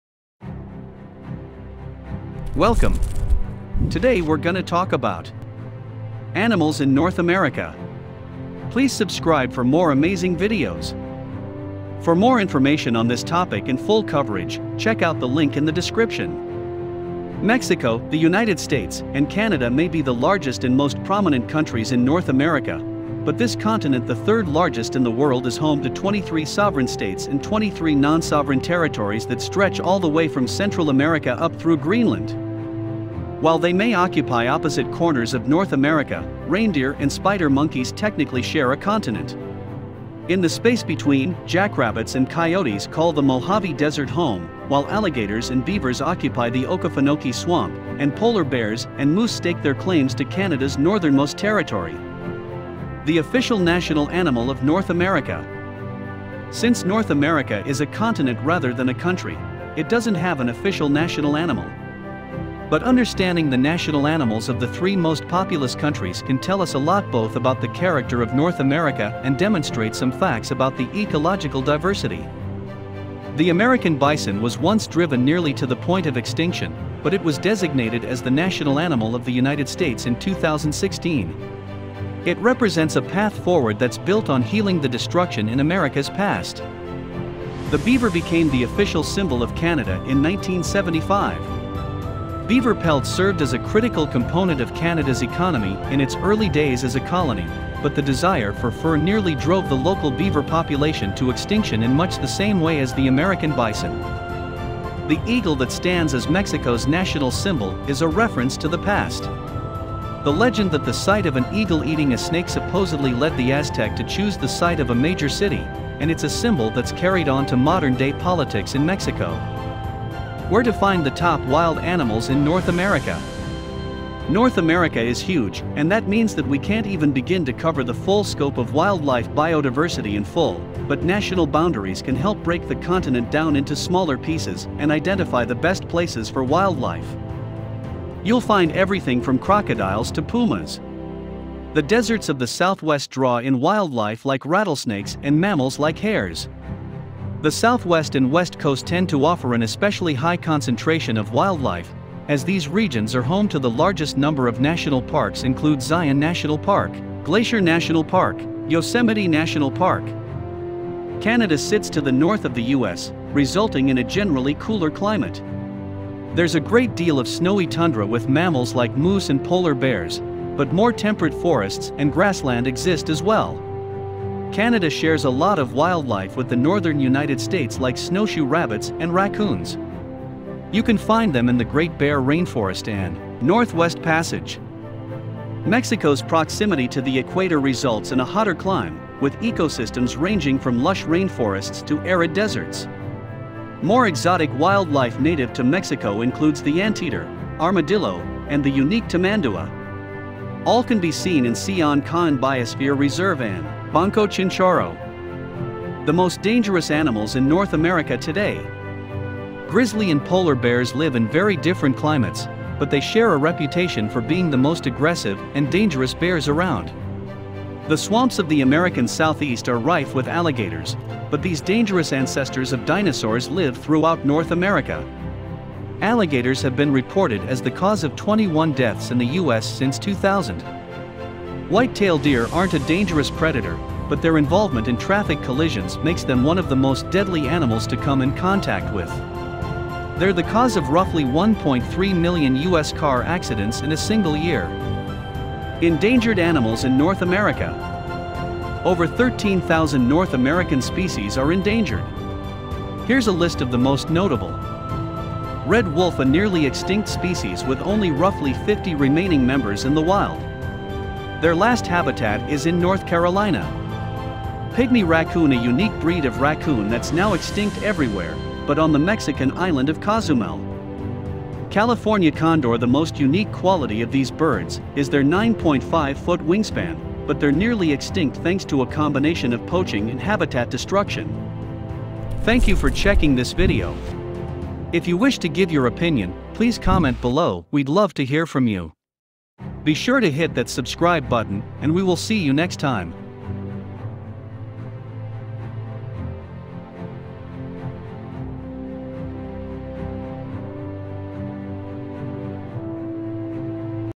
Wildlife in North America: Guide to North American Animals
Mexico, the United States, and Canada may be the largest and most prominent countries in North America, but this continent — the third largest in the world — is home to 23 sovereign states and 23 non-sovereign territories that stretch all the way from Central America up through Greenland. The sheer scope of the continent means that practically any ecosystem on Earth can be found in North America — from the snowy tundras of Greenland to the tropics of Costa Rica. Deserts, forests, mountains, and everything in-between occupy this territory along with the rich variety of animals you’d expect to find.